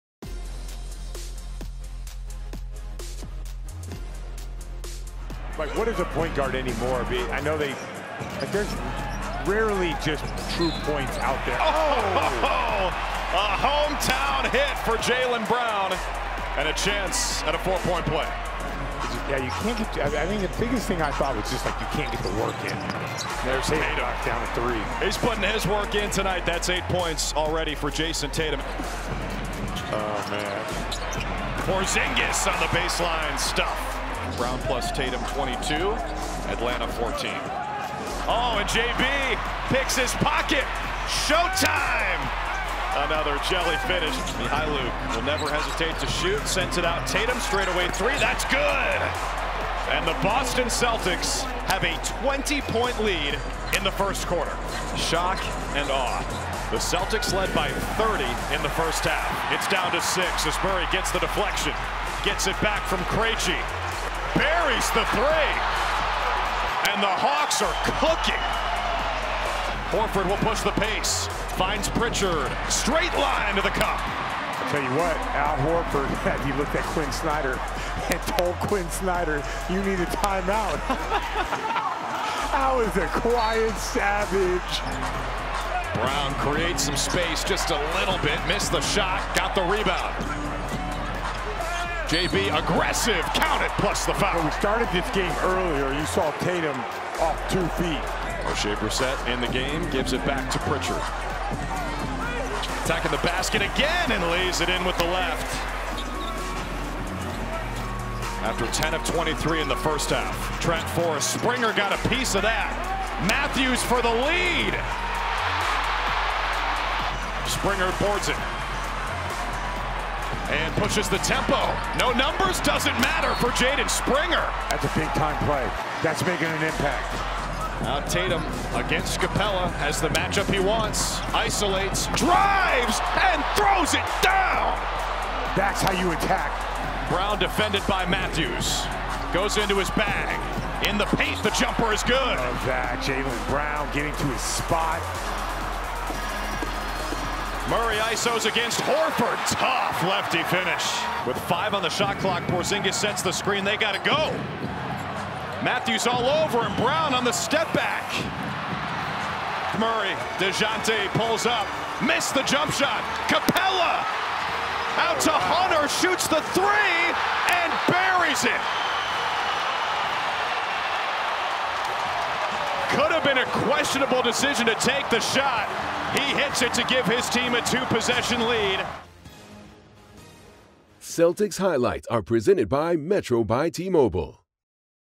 HIGHLIGHTS: Celtics lose to Hawks | Lose after leading by 30 pts for first time since 1996-97 season
Full game highlights from the Boston Celtics' 120-118 loss to the Atlanta Hawks. With the loss, Boston falls to 57-15 on the season.